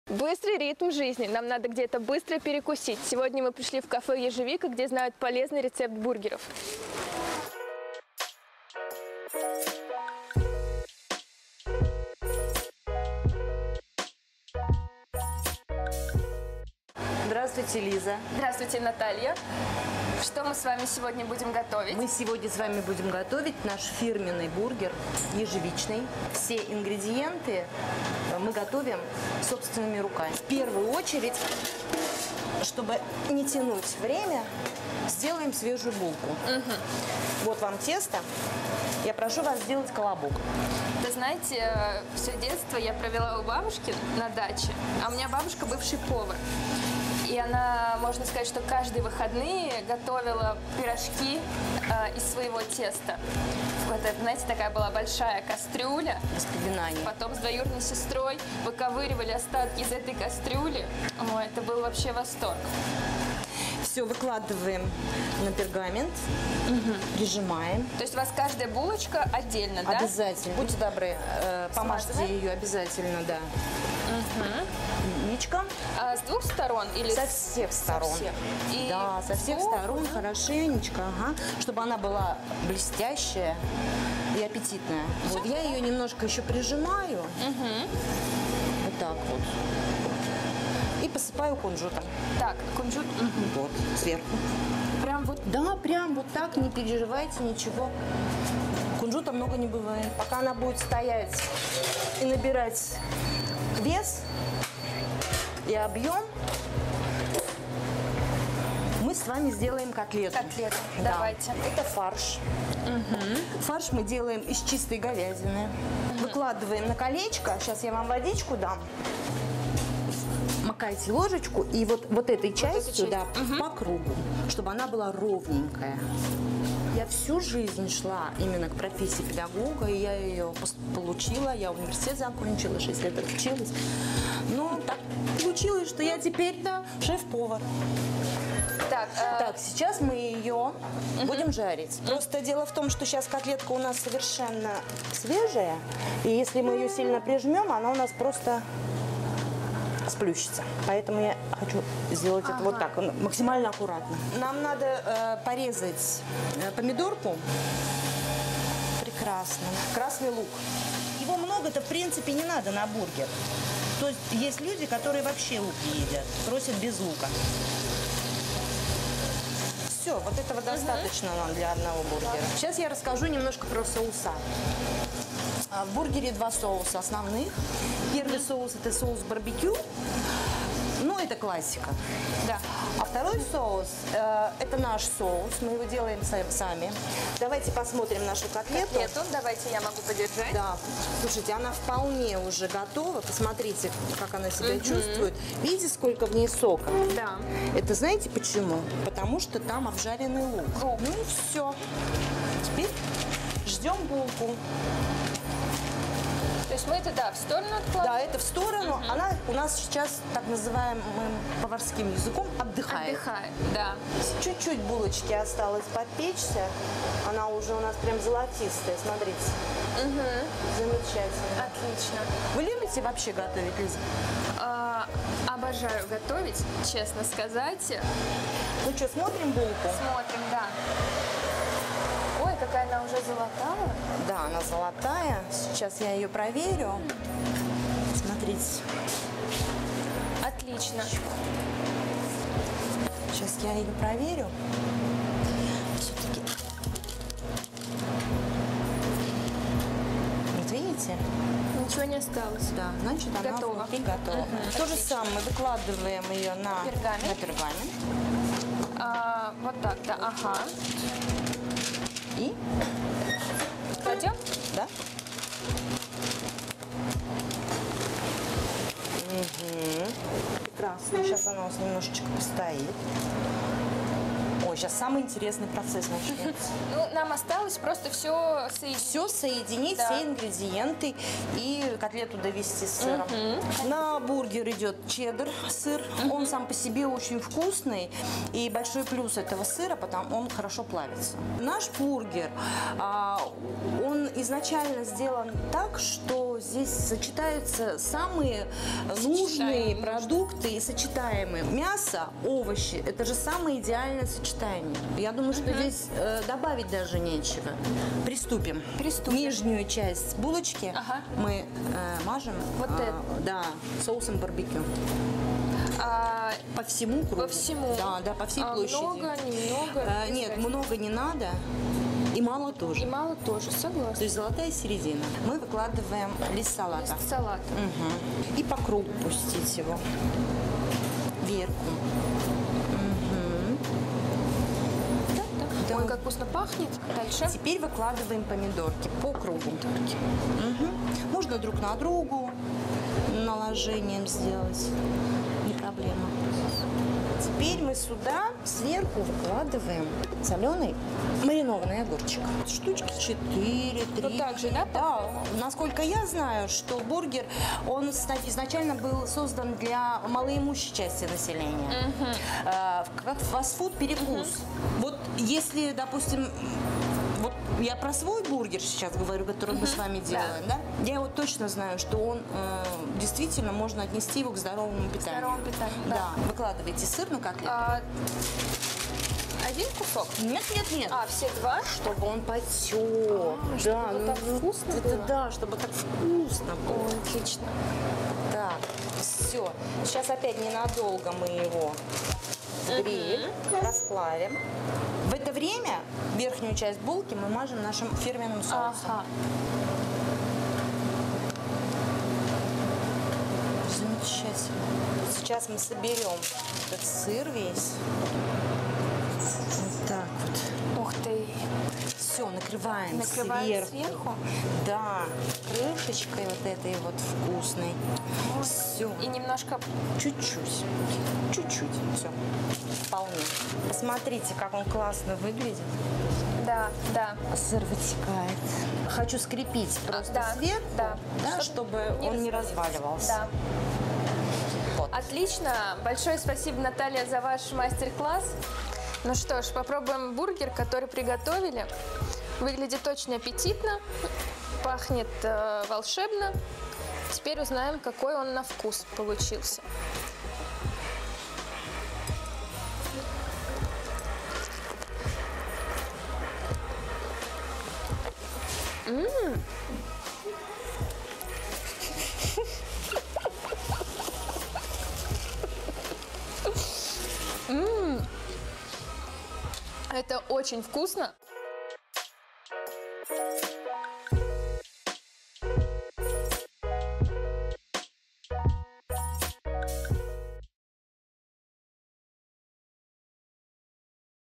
Посад на блюдечке. Полезные бургеры
При быстром ритме жизни всегда нужно знать, где можно быстро перекусить. В кафе «Ежевика» нам рассказали и показали рецепт фирменного бургера «Ежевичный».